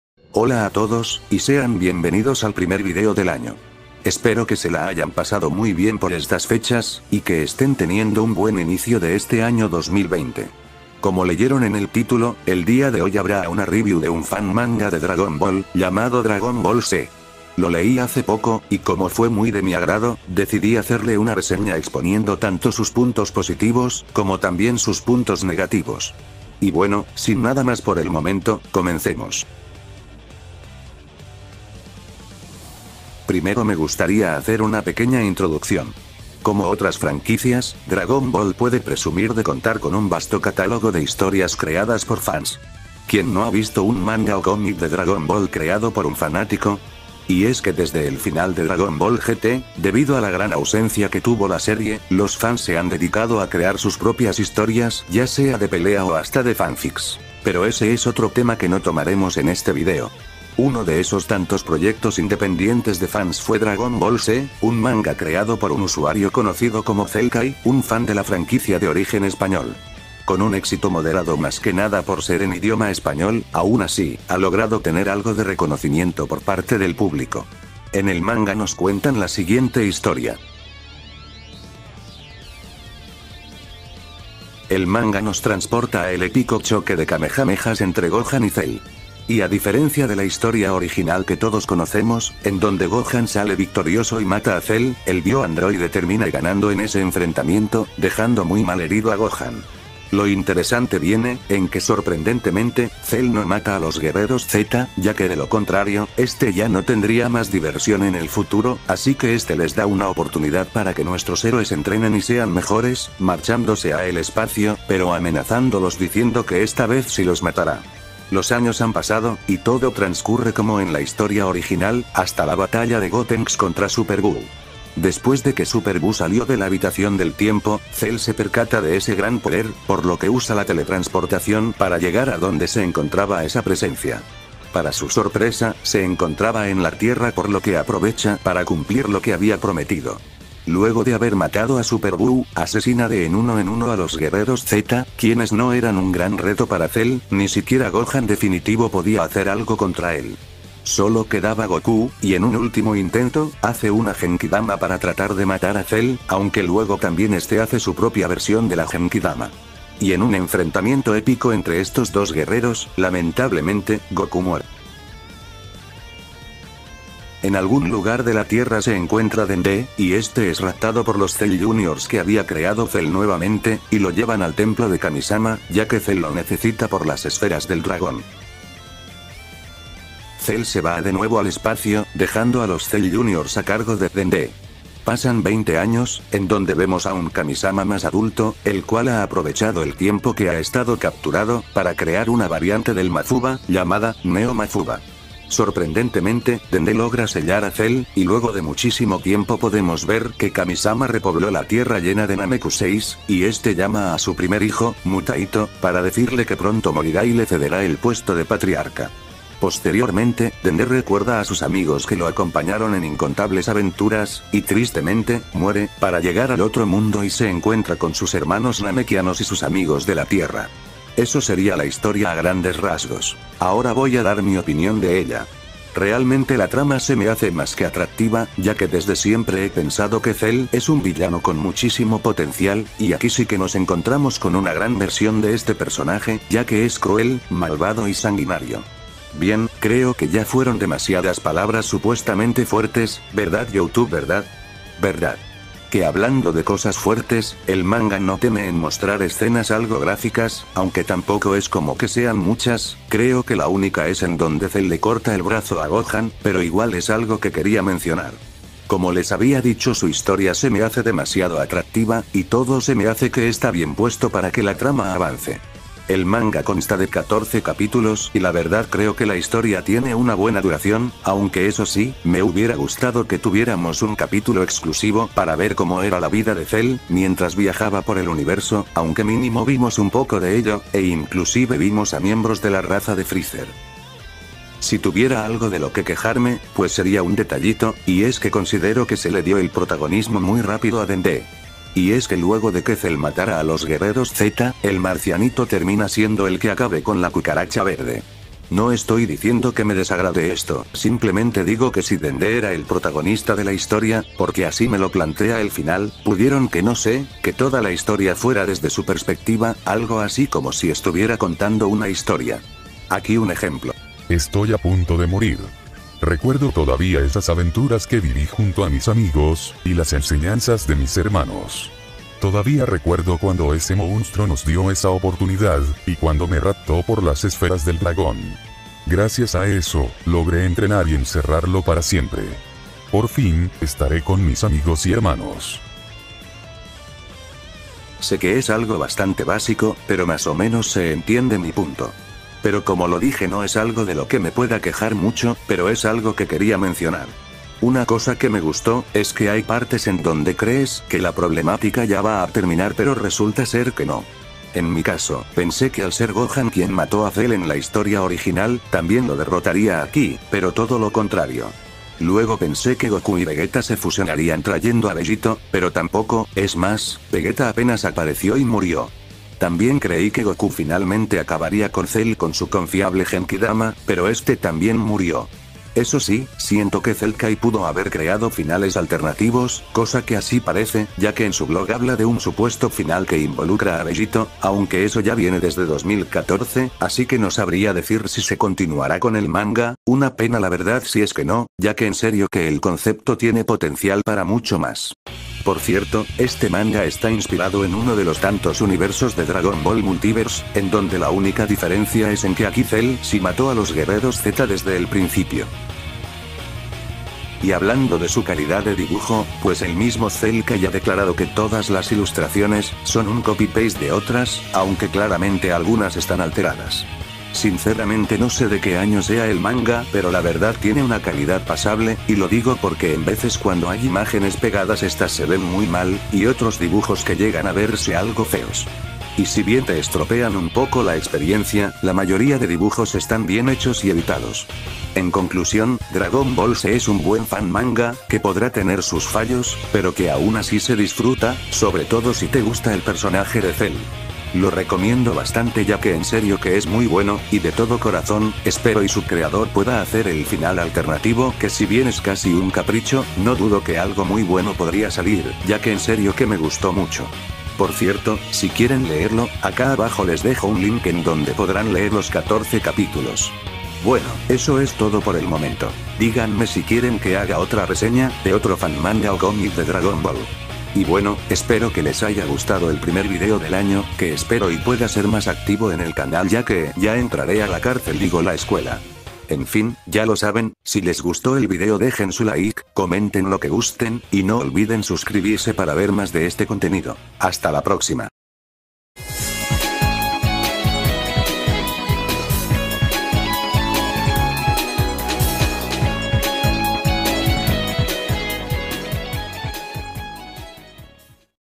Dragon Ball C|Review (Loquendo)
Una pequeña review para este fan manga
No,no me pagaron :3